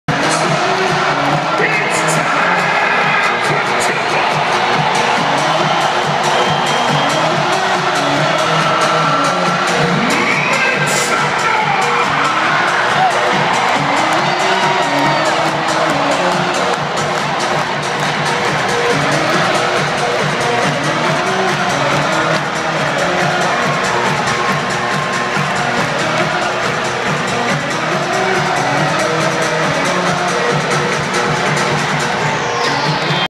Memphis Grizzlies and LA Clippers in Game 6 tip-off
Just before tip of Game 6 in the 2013 playoffs.